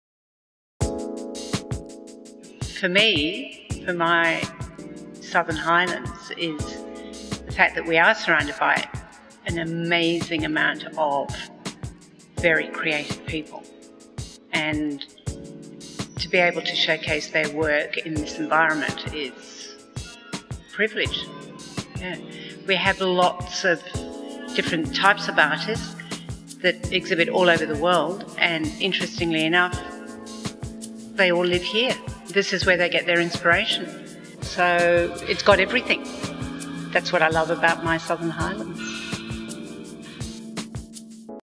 Meet the Makers Milk Factory Gallery
Shirley Becke of the Milk Factory Gallery, tells why she loves the Southern Highlands NSW. "Isn't it time you visited My Southern Highlands?"